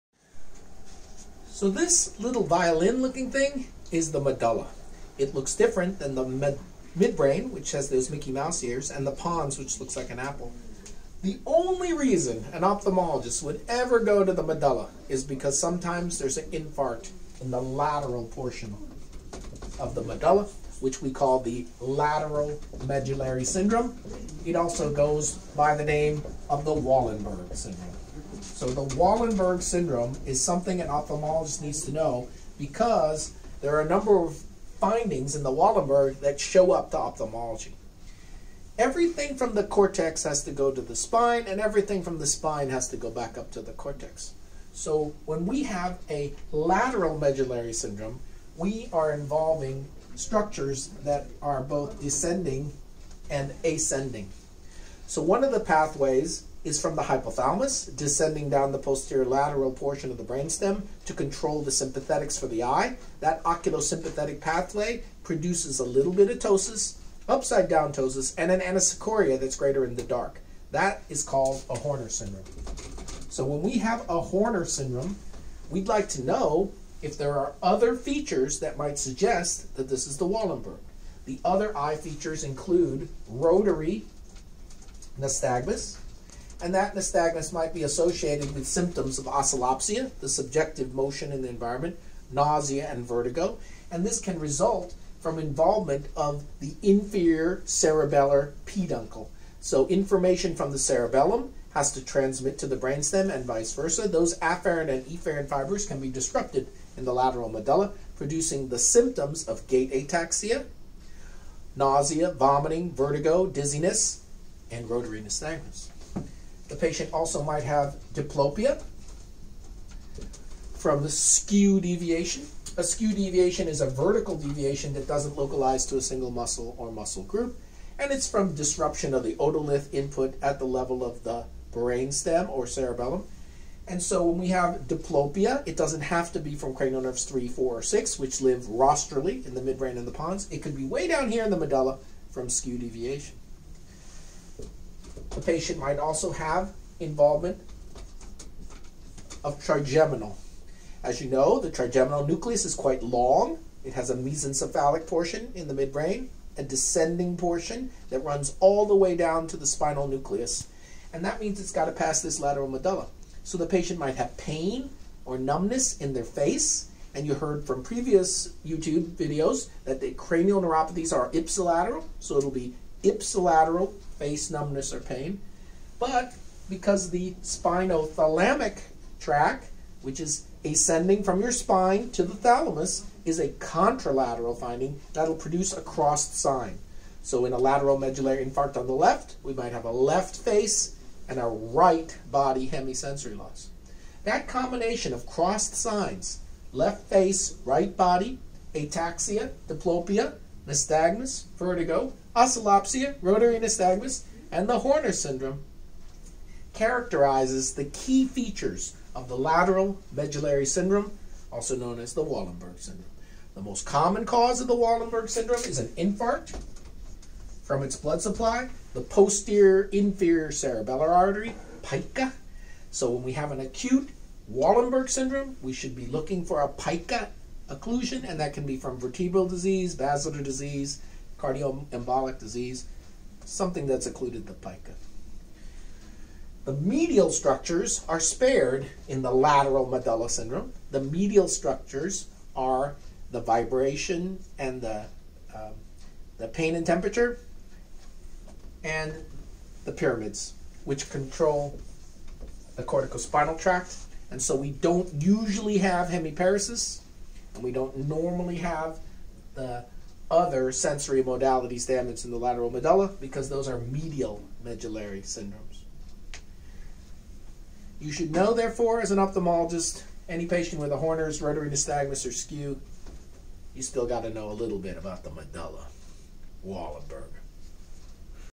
Lateral Medullary syndrome / Wallenberg syndrome
Neuro ophthalmology video # eye in lateral medullary syndrome